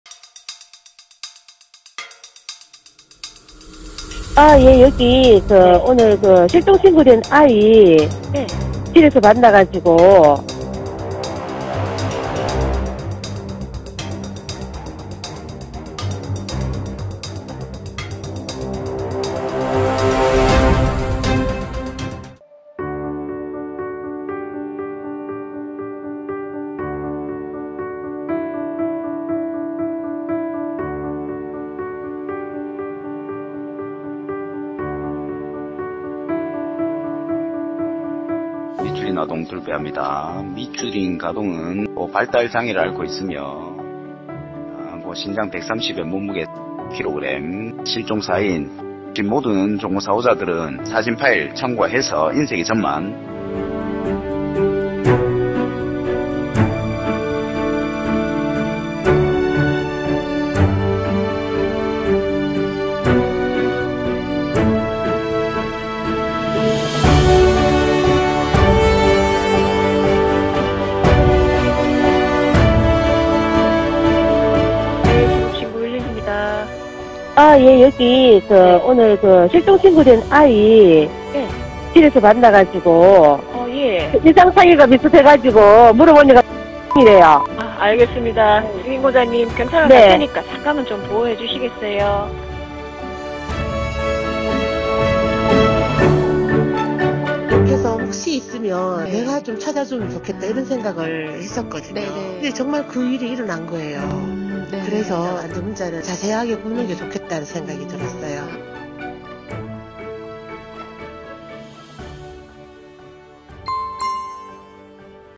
실종된 아이 시민의 신고로 찾았다!💡
우리 손녀가 없어졌어요...어쩌면 좋아...
실종된 9살 손녀를 찾으러 경찰서에 오신 어르신

이동예상경로부터 수색🚨에 들어가고,
시민의 협조를 위해 실종경보문자도 발송!

그때...!🫢

✔️ 대한민국 경찰청 공식 유튜브
구독🚨 좋아요❤️ 알림🔔 필수 💌